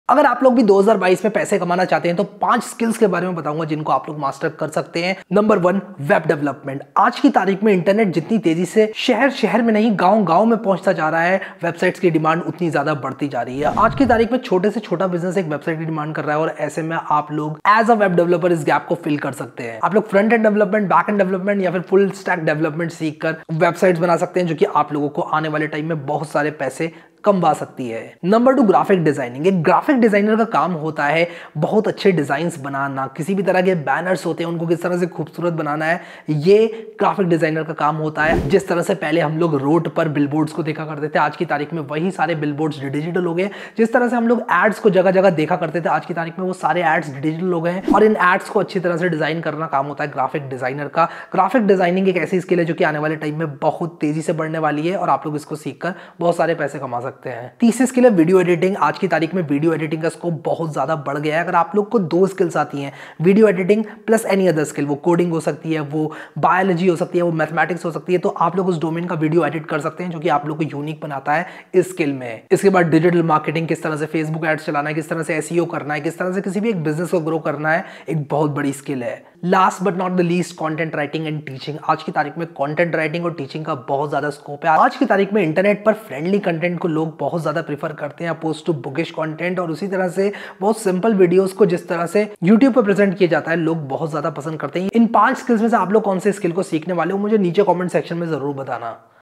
5 Top Skills to learn in 2024
In this video we will talk about 5 top skills everyone must learn in college. This video will teach you about planning to learn a new skill outside your college curriculum
All the source code and other material will be uploaded as and when available!